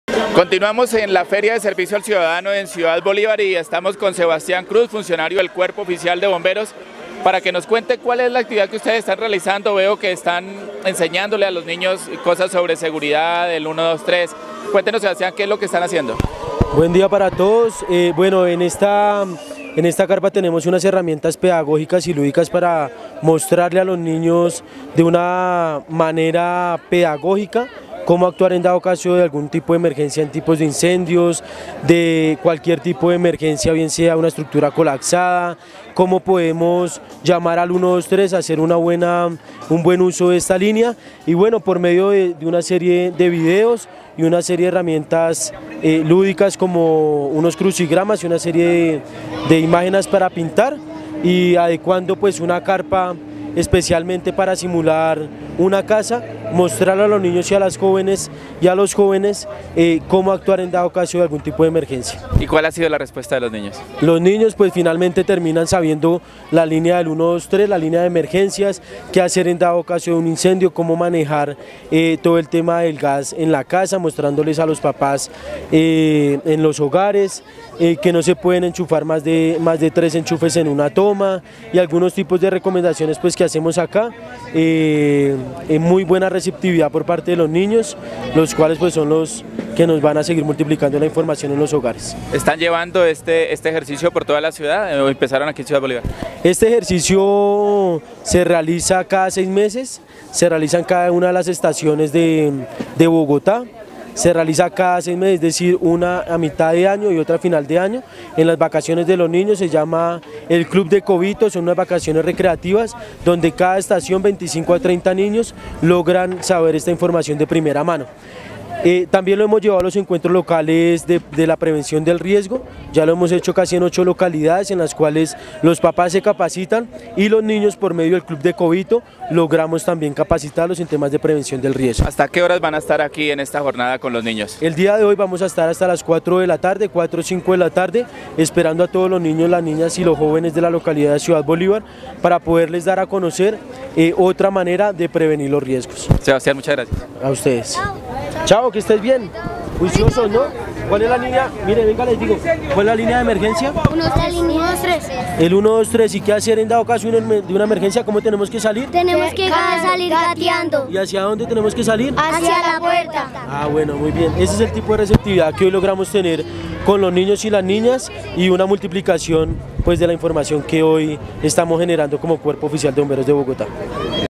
GOBERNABILIDAD HUMANA - Feria del Servicio al Ciudadano [PARTE 4]
Jornada pedagógica que tiene como finalidad explicar a los niños de Ciudad Bolívar cómo deben actuar frente a determinados casos de emergencia.